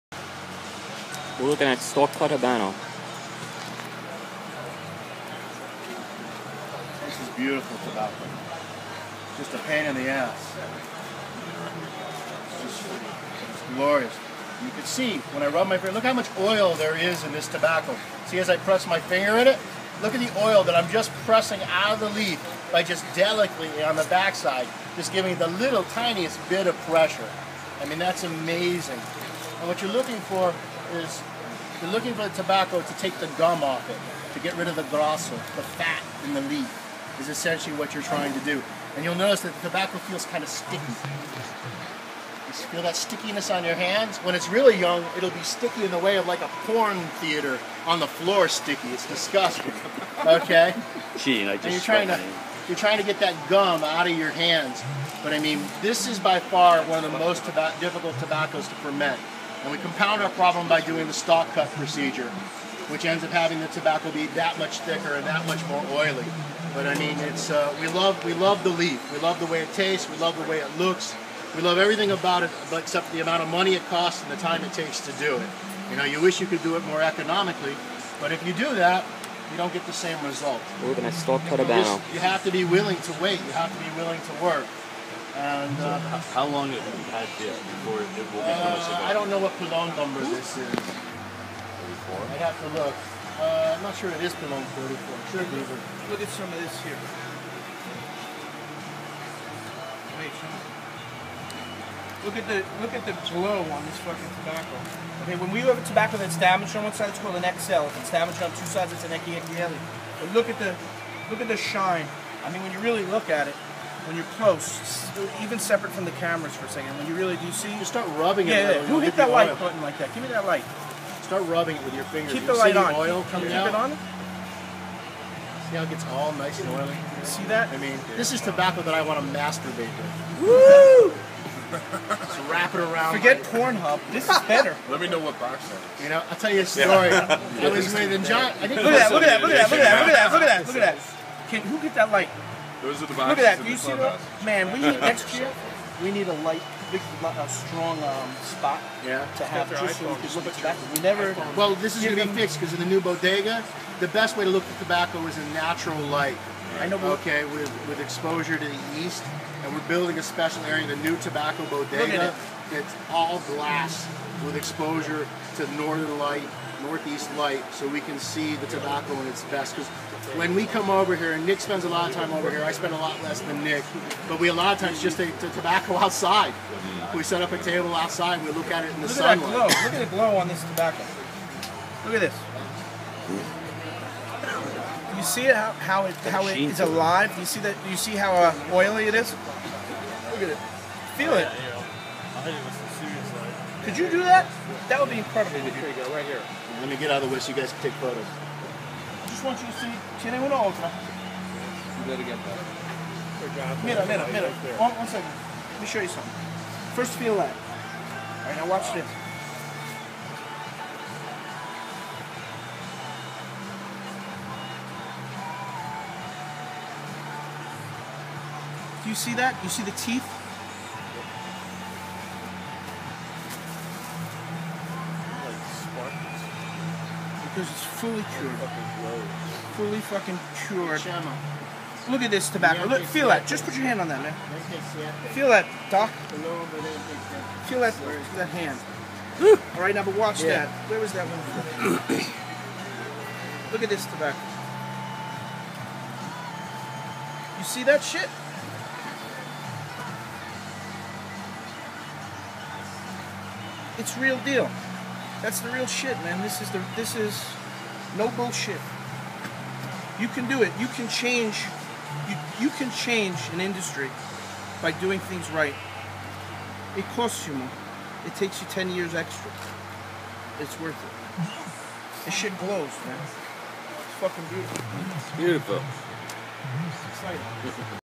Drew Estate Cigar Safari Part 25: Inspecting Some Stalk Cut Habano
In this video (filmed at the Oliva Pre-Industry facility), Jonathan Drew and Steve Saka (of Drew Estate) inspect some of the prized Stalk Cut American Habano.  They discuss the techniques and patience involved with this process.